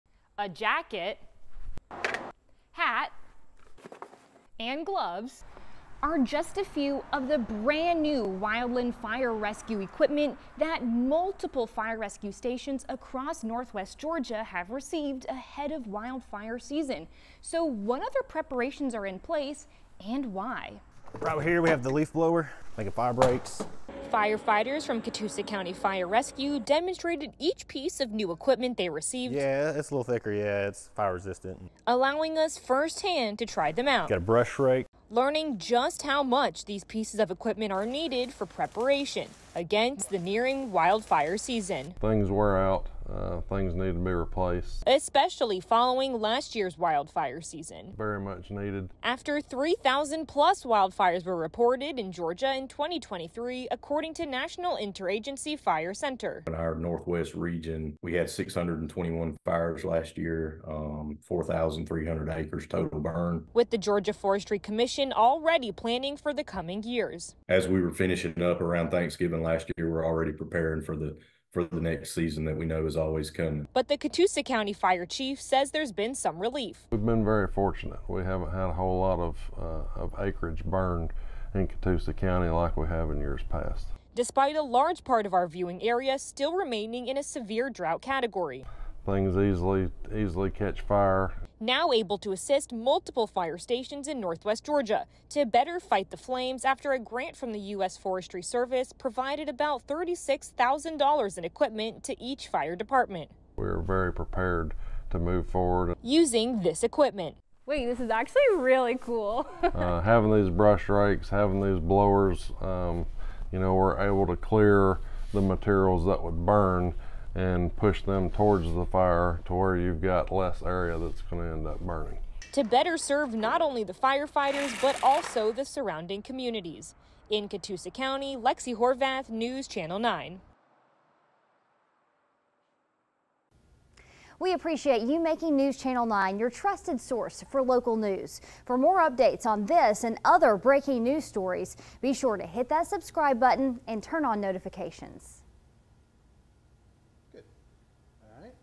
New gear fires up fire departments in Georgia amid drought dangers
Chattanooga and parts of Georgia were showered with rain for much of this past week.

But dangerous wildfires could still threaten those areas during the coming wildfire season.

The National Inter-agency Fire Center reports that Georgia experienced more than 56,000 wildfires in 2023.

Those fires burned through over 2 million acres of land and property.